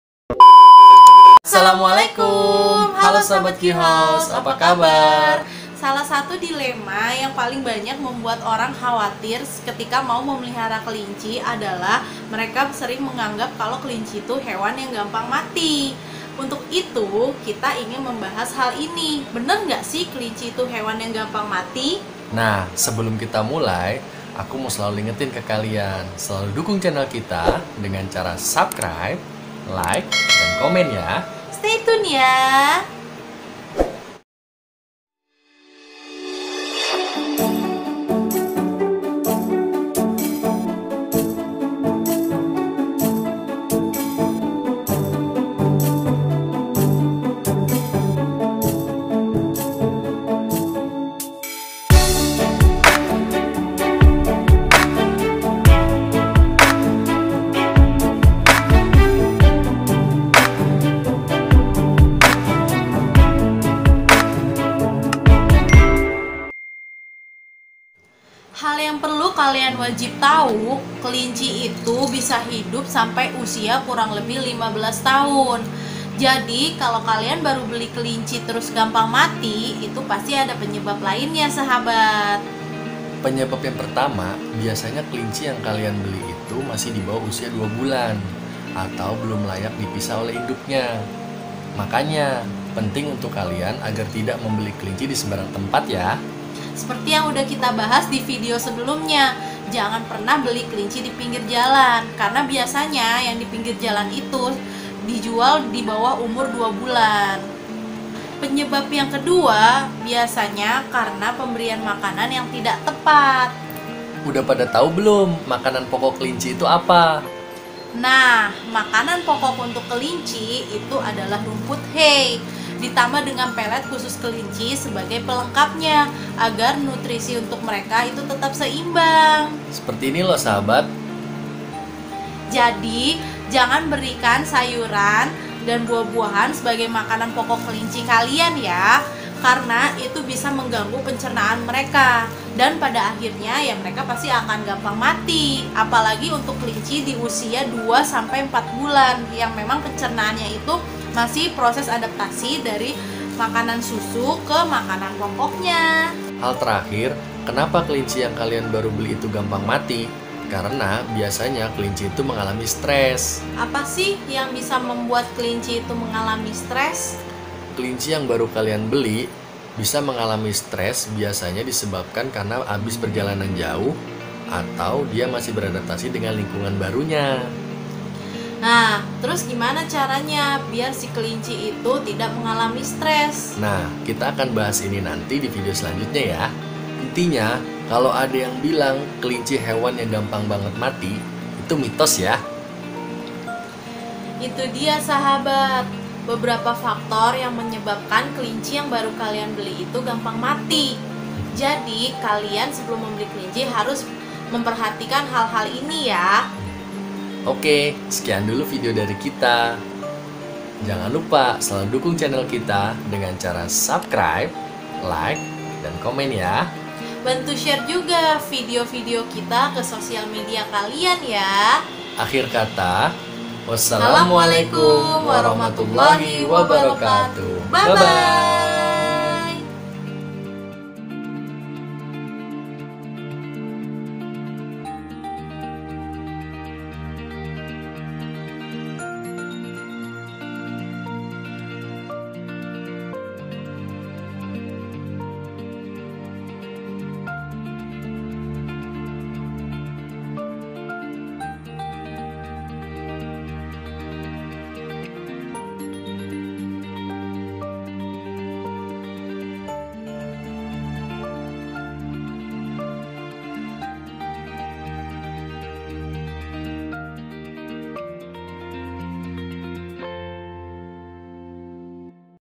Mitos Kelinci Hewan yang Gampang Mati | MITOS #2
Mitos kelinci hewan yang gampang mati Bener ga siih??
Naah di video kali ini, kita mau membahas tentang mitos kelinci hewan yang gampang mati.. penyebab utama kelinci gampang mati?  kenapa kelinci yang baru kalian beli gampang mati, dan sedikit tips agar kelinci tidak gampang mati..
Penasaran kan?? Tonton videonya sampai habis yaaa 😊